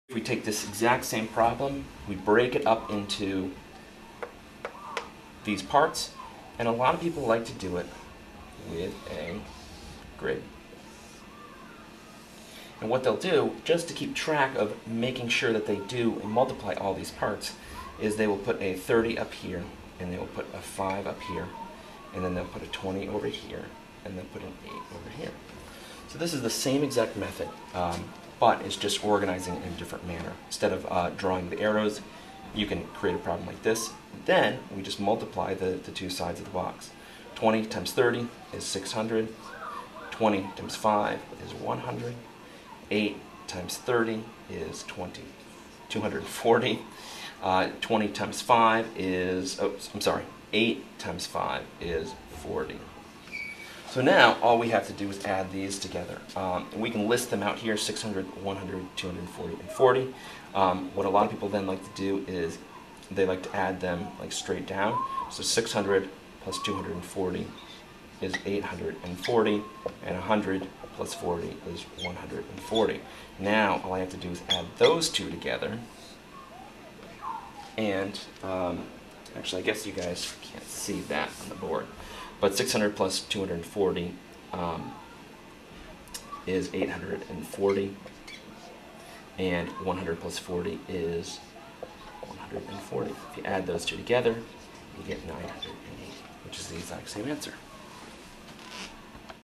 Multiplication Strategies - partial product
Here is how to multiply using the partial product method and a grid.